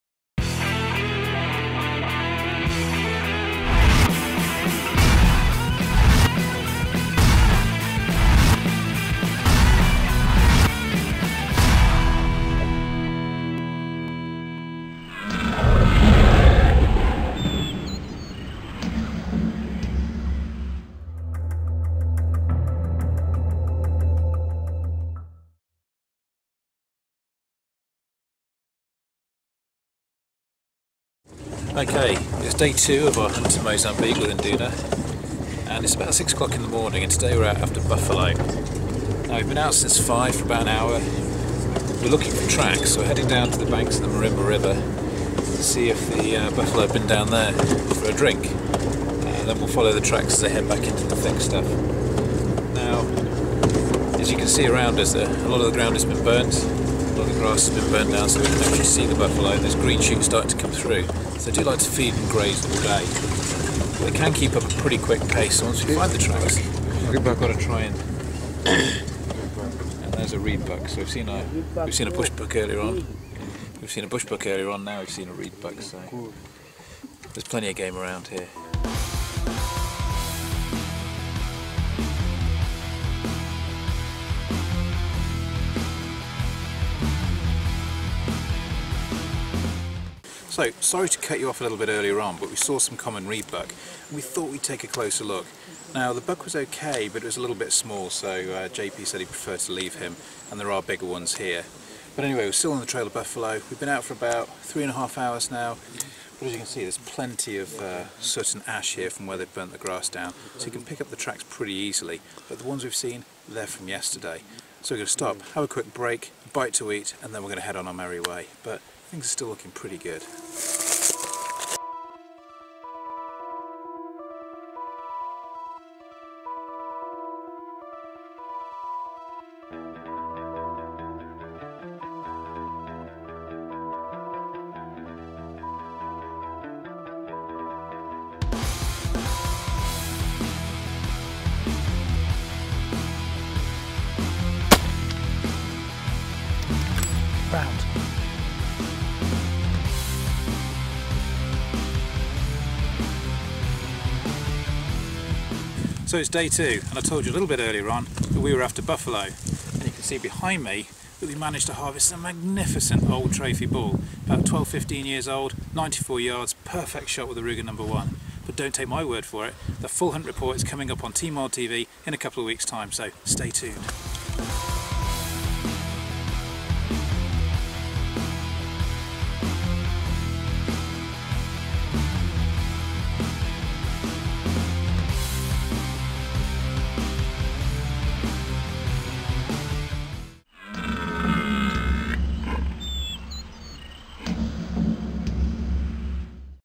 Hunting in Mozambique - Cape Buffalo!
Ian's Mozambique Vlog - Part 4. After and incredible day on the river yesterday in which we harvested a magnificent trophy crocodile, we have no plans to ease the pace. This trip is all about one animal - the formidable 'black death' of the African veld - the Cape Buffalo. Regarded by most experienced African hunters to be the most dangerous animal on the planet, the cape buffalo is no easy animal to stalk, let alone harvest. We'll need all our wits and skill to overcome this adversary.